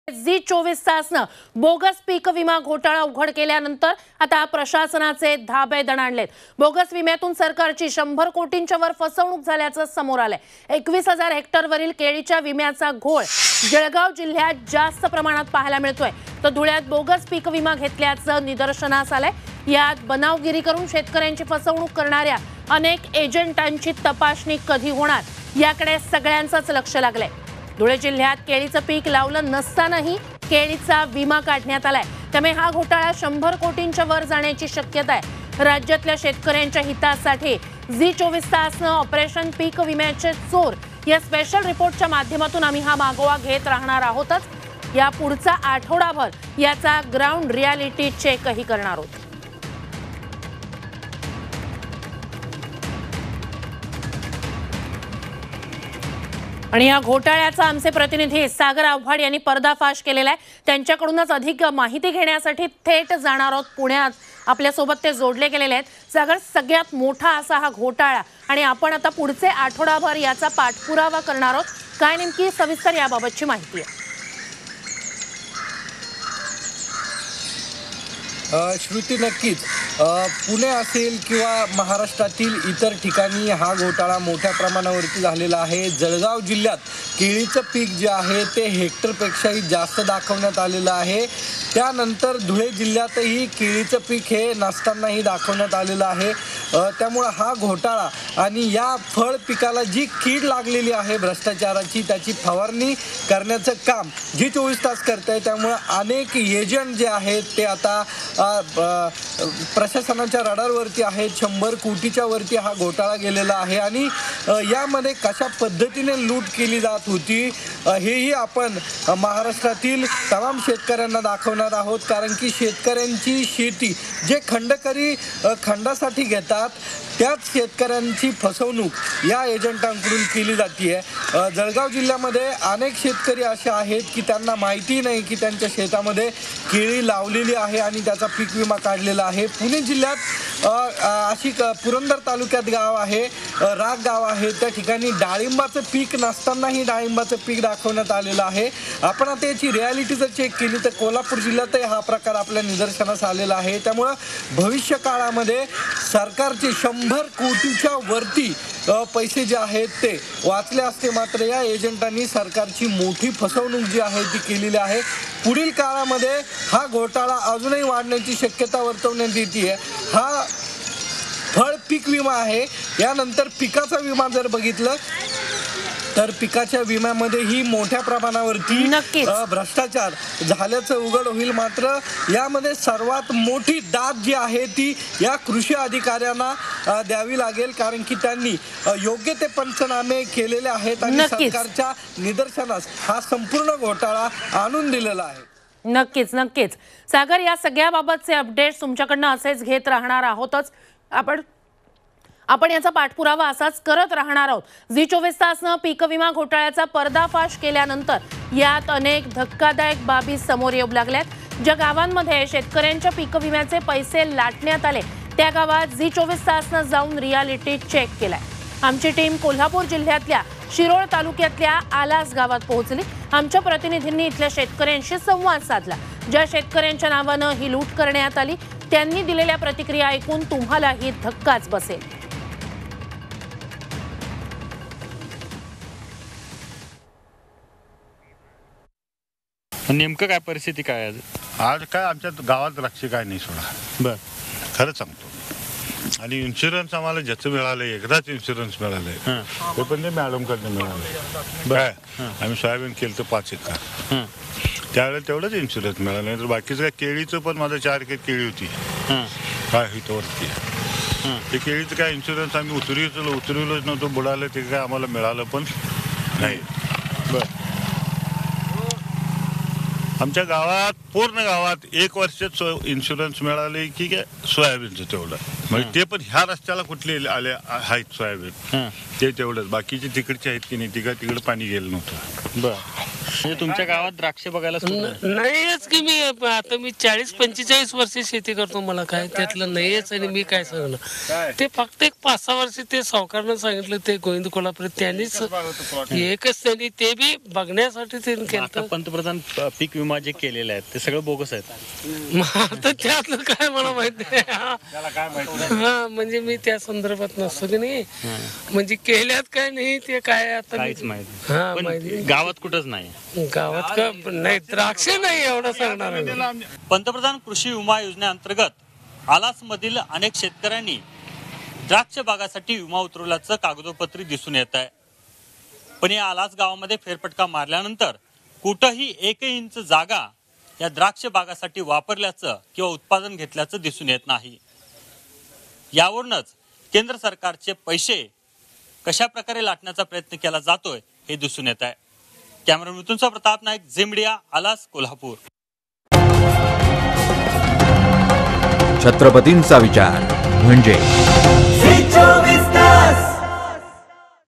सर्वात मोठी बातमी! 100 कोटींची फसवणूक; शेतकऱ्यांच्या पिक विम्याचे पैसे लाटतंय कोण?
झी २४ तासने बोगस विमा घोटाळा उघड केल्यानंतर प्रशासनाचे धाबे दणाणलेयत...बोगस विम्यातून सरकारची शंभर कोटींच्यावर फसवणूक झाल्याचं समोर आलंय... 21 हजार हेक्टर वरील केळीच्या विम्याचा घोळ जळगाव जिल्ह्यात जास्त प्रमाणात पाहायला मिळतोय...तर धुळ्यात बोगस पीक विमा घेतल्याचं निदर्शनास आलंय...यामध्ये बनावगिरी करून शेतकऱ्यांची फसवणूक करणाऱ्या अनेक एजंटांची तपासणी कधी होणार...?  याकडे सगळ्यांचे लक्ष लागलंय...तर धुळे जिल्ह्यामध्ये केळीचे पीक लावलं नसतानाही केळीचा विमा काढण्यात आलाय...त्यामुळे हा घोटाळा 100 कोटीच्या वरती जाण्याची शक्यता आहे...महाराष्ट्रातल्या शेतक-यांच्या हितासाठी झी २४ तास ऑपरेशन पीकविम्याच्या चोर या स्पेशल रिपोर्टच्या माध्यमातून मागोवा घेतच राहणार...आम्ही पुढचा आठवडाभर याचा ग्राऊंड रिऍलिटी चेक करणार आहोत...

After Zee 24 Tas revealed the bogus insurance scam, the administration has been shocked... It has come to light that the government has been defrauded of Rs. ...When will the many agents who are cheating the farmers by forgery be investigated...?Everyone's attention has been drawn to this...in Dhule district even though banana crop has not been planted, banana insurance has been taken out...therefore this scam is likely to go above 100 crores...Zee 24 Hours Operation Crop Insurance Thieves for the benefit of Maharashtra farmers is tracked through this special report. Stay...We will be doing a ground reality check on this for the next week...

Social Media Links :
5 Minitat 25 Batmya । 5 मिनिटांत 25 बातम्या